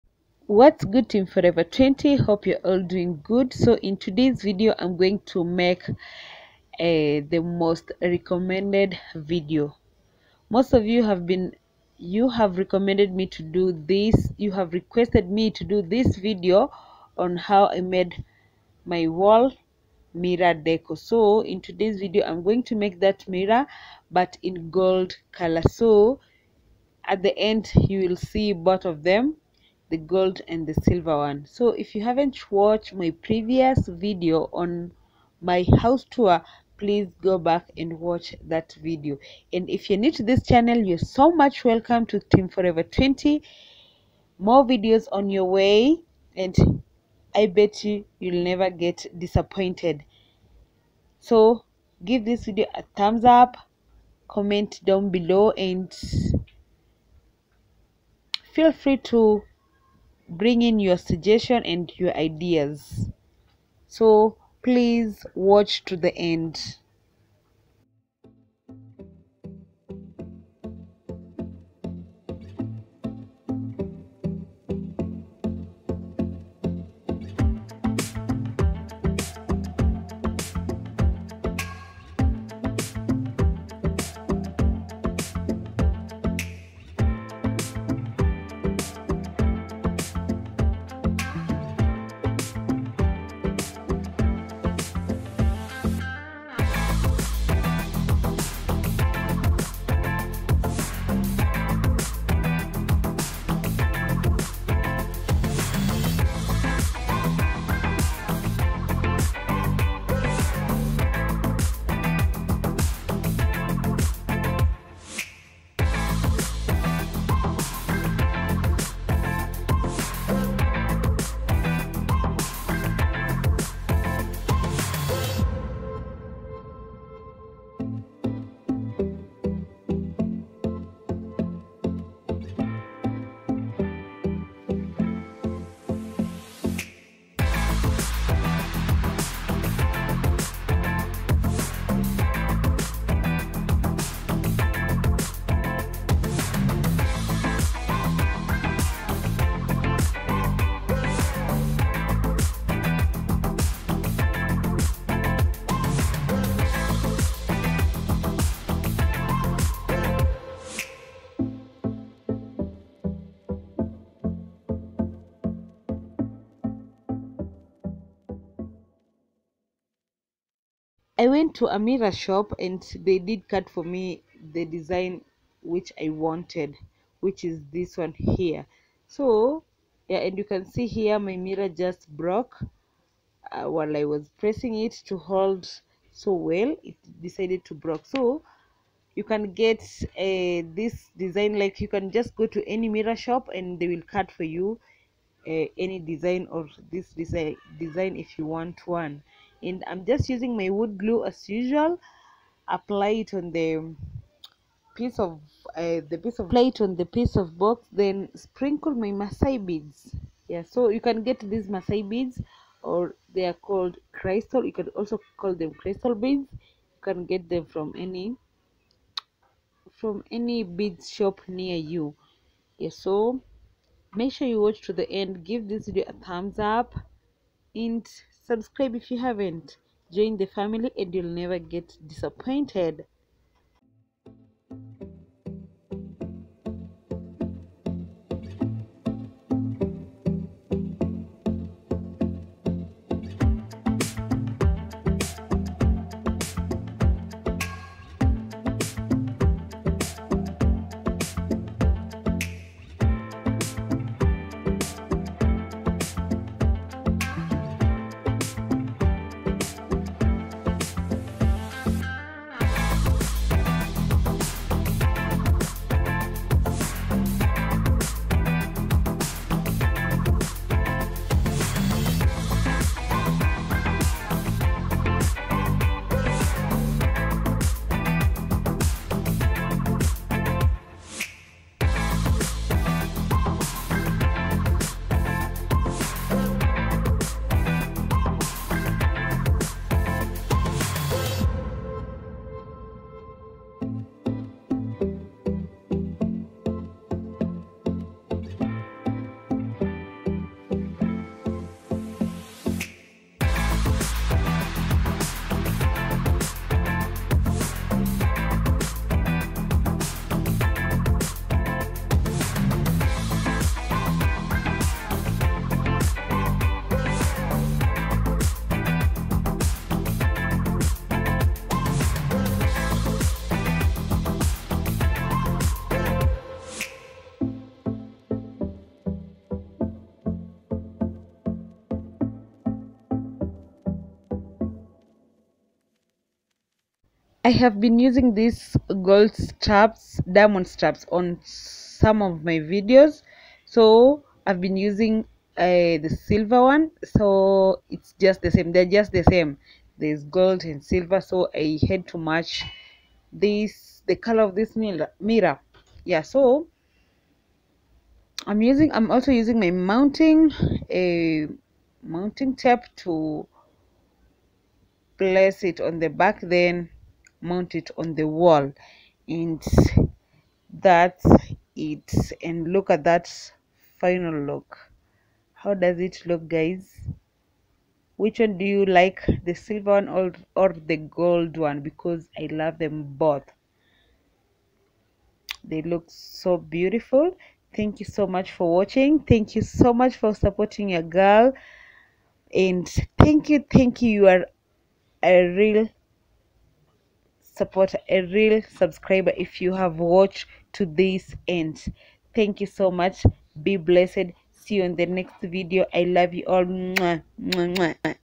DIY WALL MIRROR DECOR // high end wall mirror decor.
Hello Team Forever20.

Today's video am showing you how to make a high end mirror decor.

materials used:
Outer mirror 50cm (diameter)
Inner mirror 31cm
wood glue
conta glue
glue gun
crystal beads
spray paint
Double sided mounting tape
Diamond straps

PEACE ✌